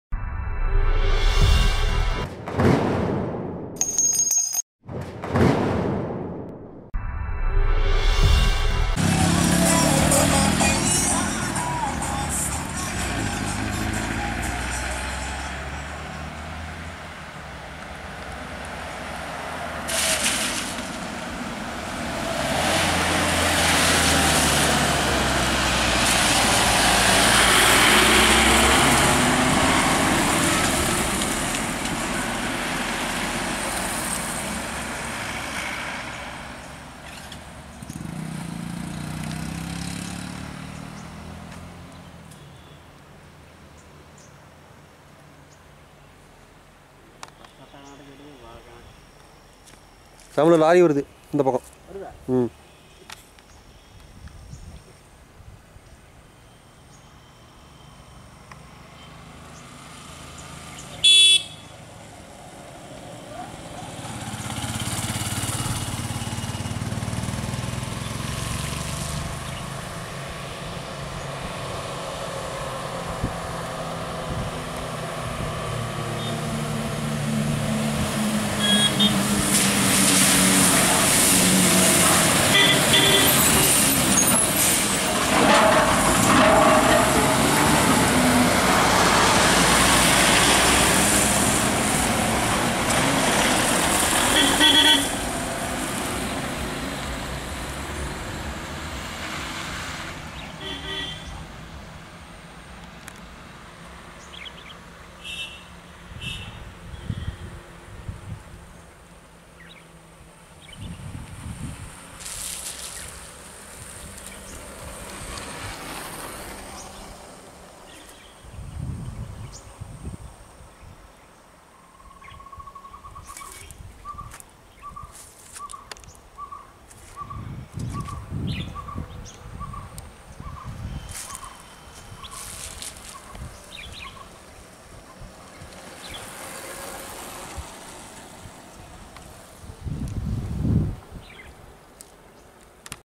Kodaikanal Hills: Ashok Leyland Gas Lorry Turning Kodaikanal Hairpin Bend Road at Dindigul.
Kodaikanal Hills:
                                   Ashok Leyland Gas Lorry Turning Kodaikanal Hairpin Bend Road at Dindigul.